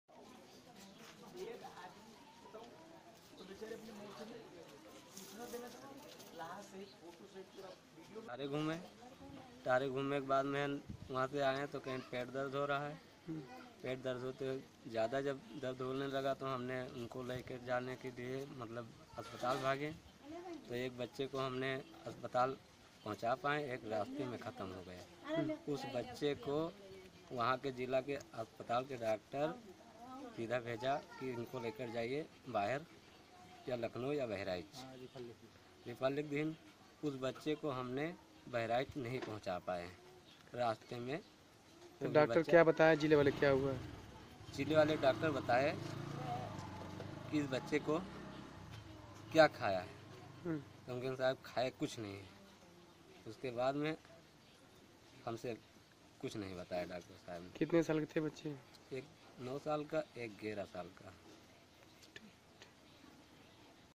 फूड प्वाइजनिंग से दो बच्चों की मौत, खबर सुनते ही सदमे से दादा ने तोड़ा दम
श्रावस्ती : दो बच्चों को पेट में दर्द की शिकायत के बाद उल्टियां शुरू हो गईं. इन्हें पिरजन इलाज के लिए संयुक्त चिकित्सालय लेकर भागे. वे रास्ते में ही थे. इस दौरान एक बच्चे ने दम तोड़ दिया, जबकि दूसरे बच्चे को चिकित्सकों ने लखनऊ रेफर कर दिया.